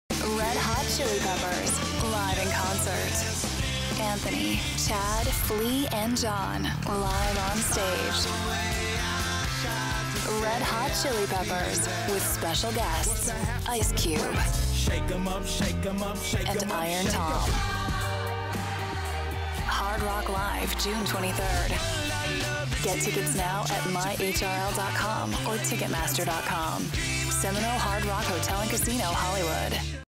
Red Hot Chili Peppers at Hard Rock Live - June 23
Get your tickets now to see Red Hot Chili Peppers at Hard Rock Live on Sunday, June 23!